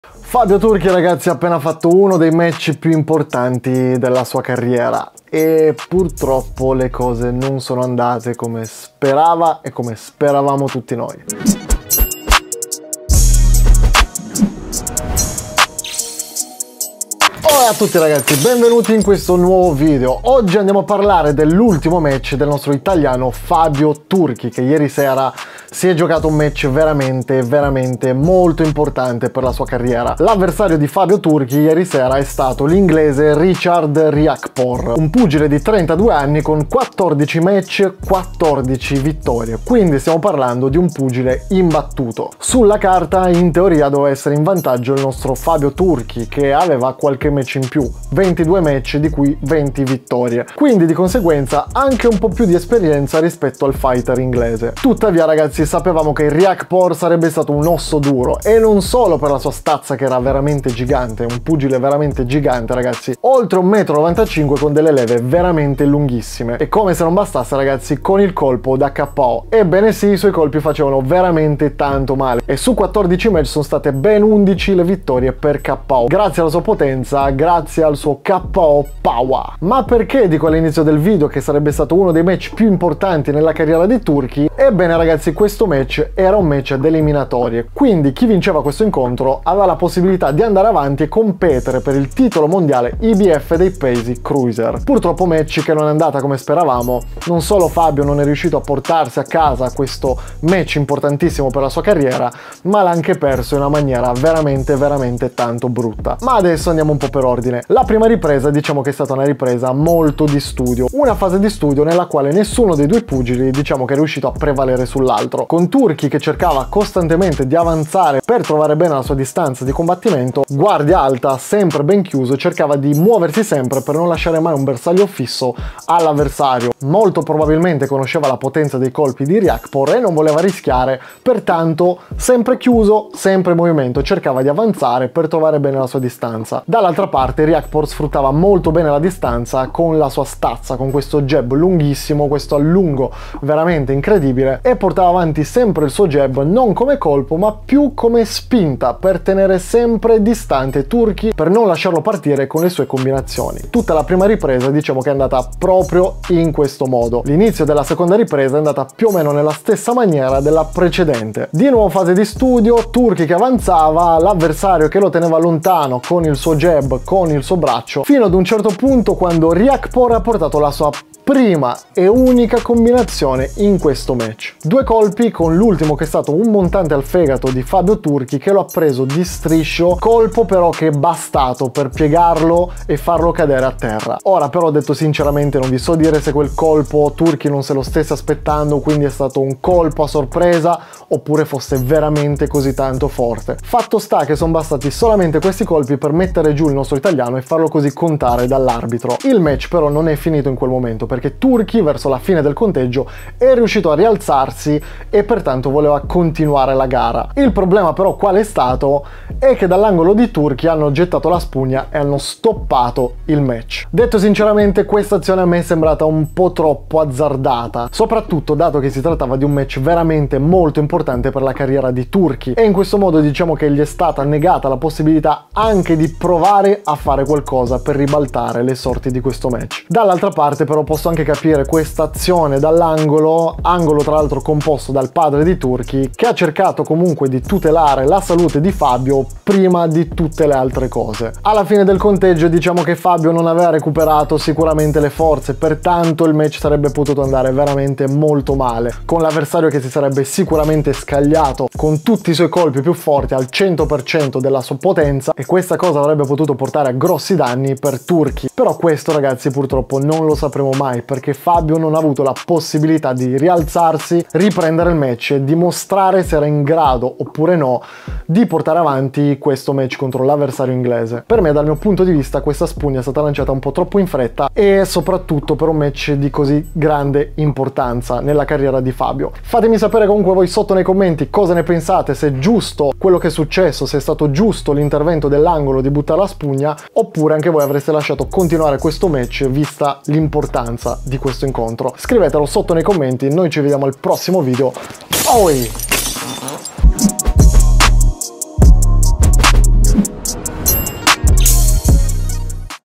TURCHI vs RIAKPORHE | Tremendo KO! Giusto il RITIRO?
Sabato scorso Fabio Turchi ha avuto un match davvero importante per la sua carriera contro l'avversario inglese, il pugile Richard Riakporhe, match che avrebbe permesso di continuare la sua scalata verso il titolo mondiale IBF dei pesi Cruiser. Purtroppo l'avverario ha avuto la meglio nella seconda ripresa, con un ritiro di Fabio Turchi un po prematuro...